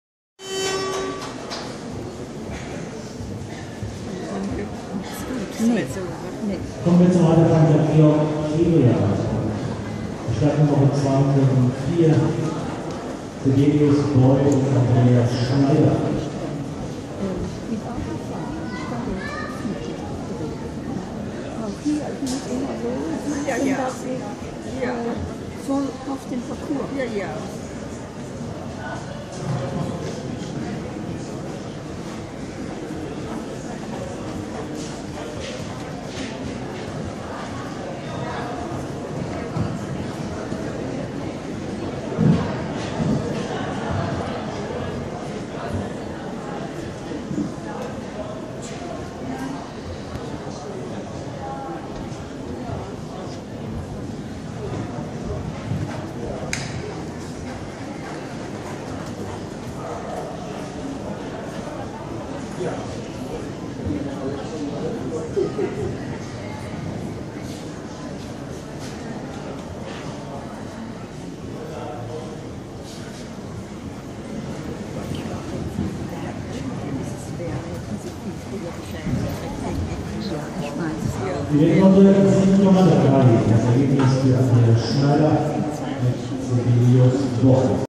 Stil-A* in Xanten mit Cebi
Wertnote 7,3
Ab 7,4 wurde platziert, schade aber trotzdem zufrieden.